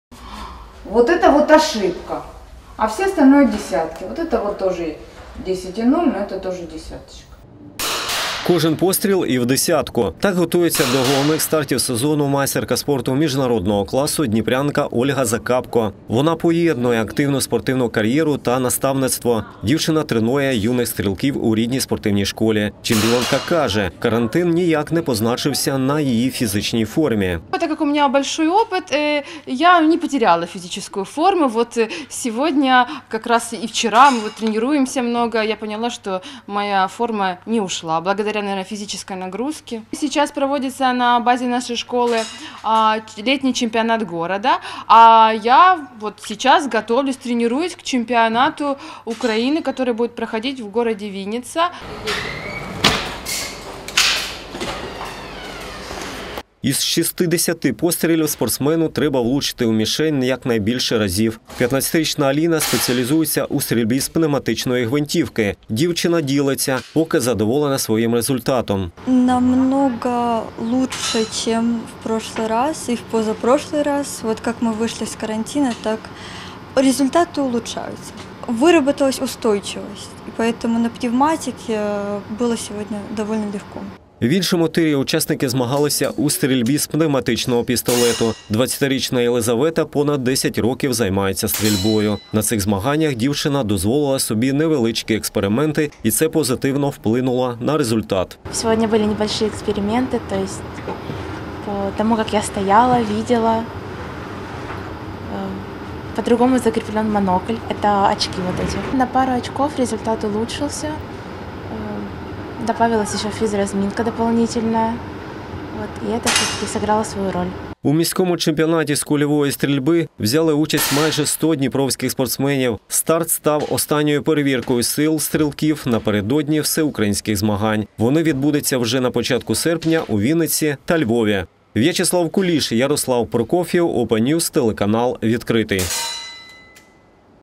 Стрільці Дніпра набирають спортивної форми
Вже за тиждень на них чекають Всеукраїнські змагання, які пройдуть у Вінниці та Львові. Наскільки спортсмені готові до відповідальних стартів визначив літній міський чемпіонат з кульової стрільби. Цього разу на старт змагань у тирі місцевої СДЮСШОР № 1 вийшли і юнаки і дорослі учасники